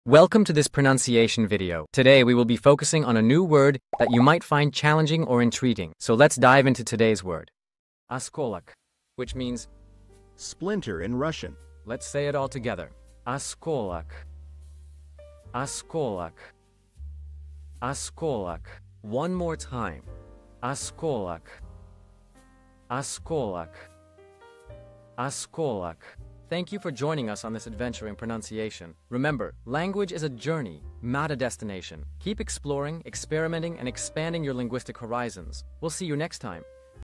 How to pronounce Oskolok | Осколок (Splinter in Russian)
Master the Pronunciation of 'Oskolok | Осколок' - which means : Splinter in Russian 📝 with @PronunciationRussian 🎓🗣️ - [Your Guide to Russian]

Learn to pronounce 'Oskolok' perfectly in Russian with our easy and engaging tutorial by @PronunciationRussian! 📖📽️ In this comprehensive video, we provide you with native speaker examples 🗣️👂 and phonetic breakdowns 🎵 to ensure you get the pronunciation just right.